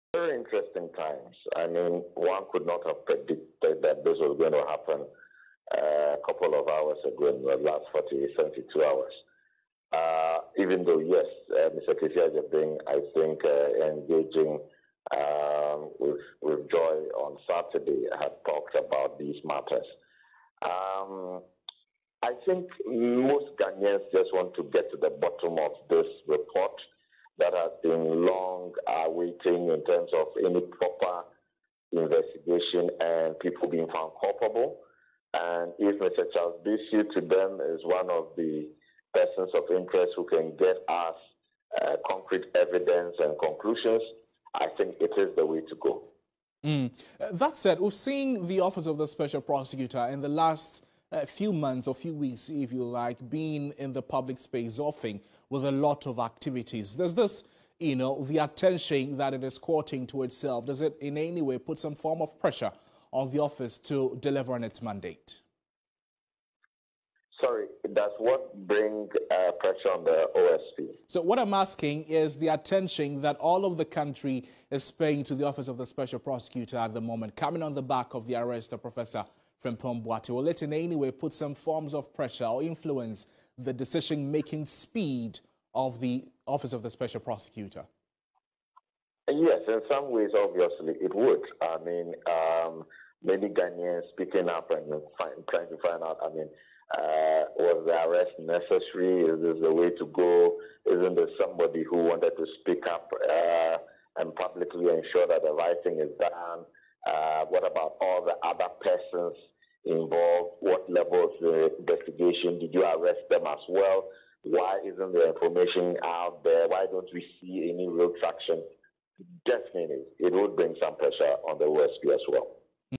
Office of the Special Prosecutor declares Charles Bissue wanted.
Charles Bissue has been declared wanted by the Office of the Special Prosecutor on corruption and corruption-related offences regarding the activities and expenditure of the dissolved Inter-Ministerial Committee on illegal Mining.